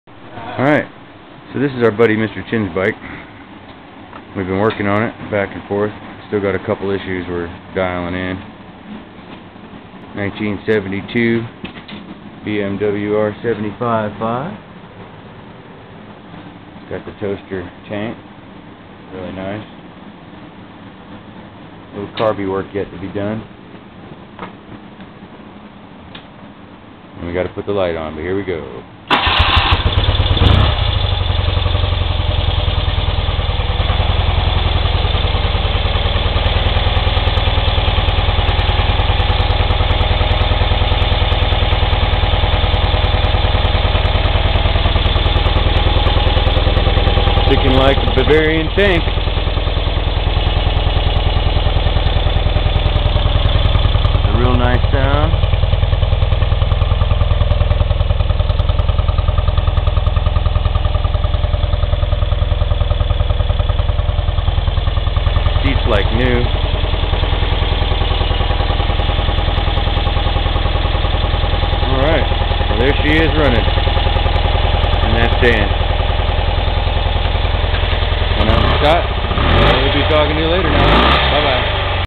Mr. Chins BMW running
1972 BMW R75/5 with a toaster tank.. finally running.. got a leak issue with the float bowls on the carbs,, probably just a couple of gaskets...I'll sync her up, detail her, and DONE!!